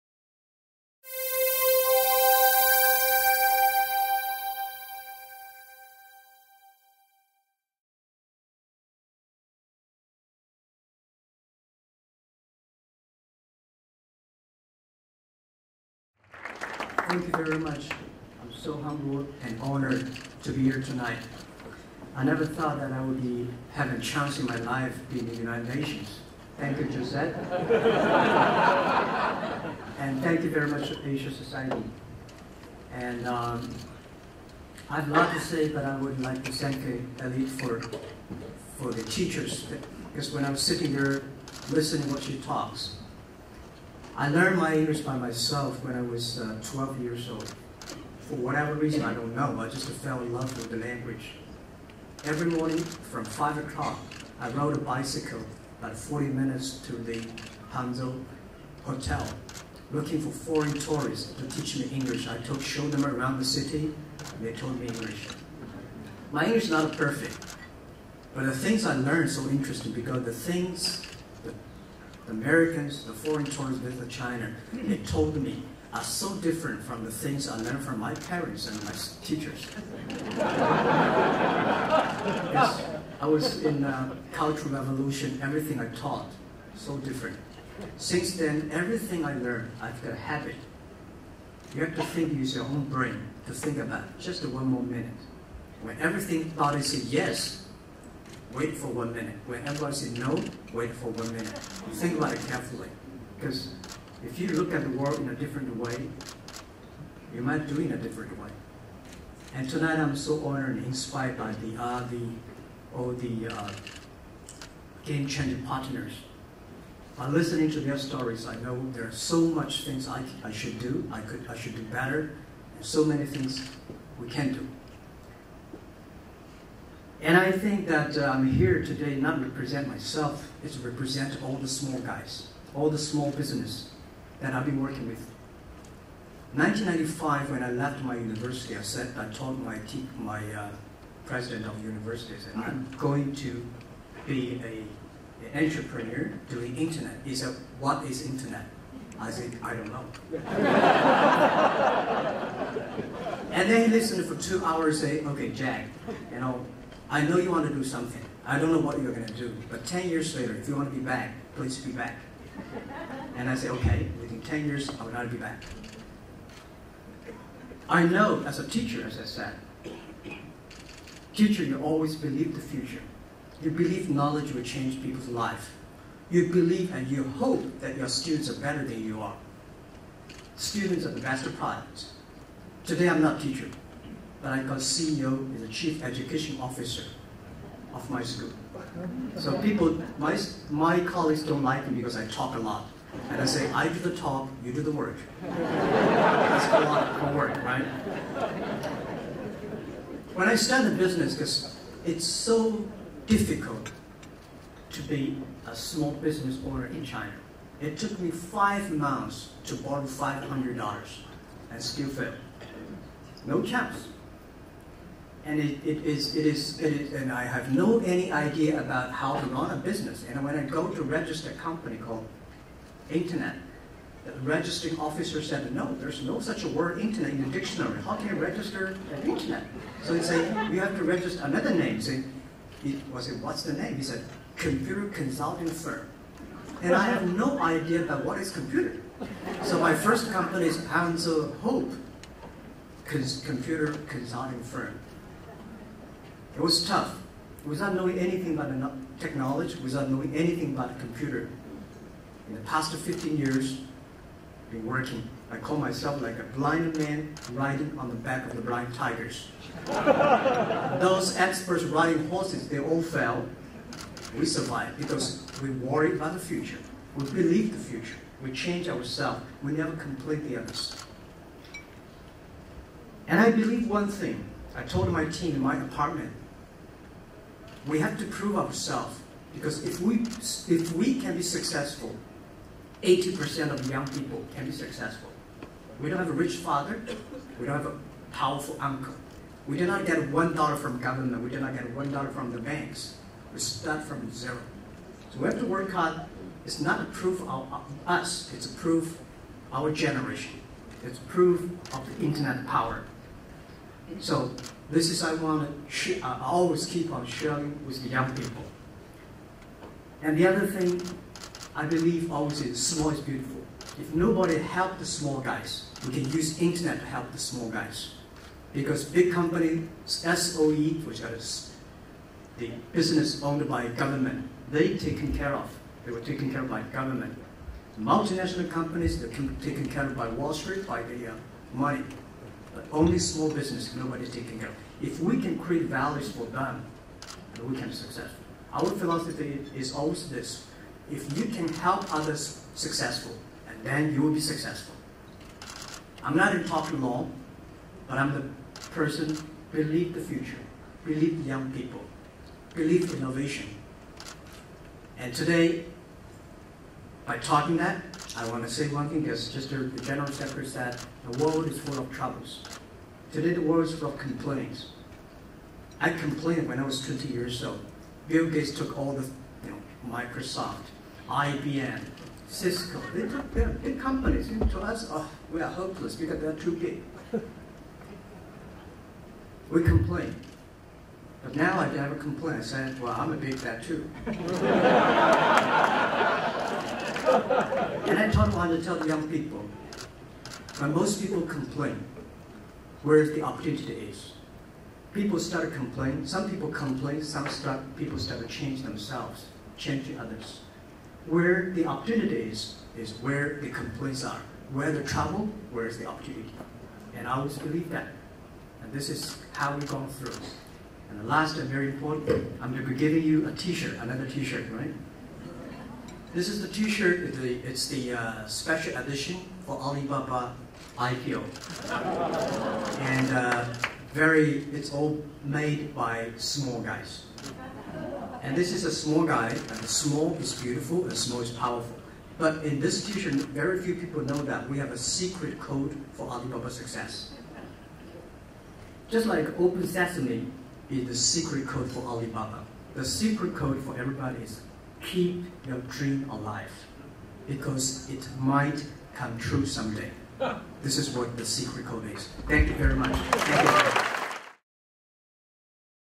Jack Ma: 'Small Is Beautiful, Small Is Powerful'
NEW YORK, October 16, 2014 — Alibaba founder and CEO Jack Ma accepts his Asia Game Changer of the Year award from Asia Society in a ceremony at the United Nations. Ma recounts his company's rise from modest beginnings and concludes with the homily, "Keep your dream alive because it might come true one day."
Photos: Asia Society Honors Jack Ma, Other 'Asia Game Changers' at United Nations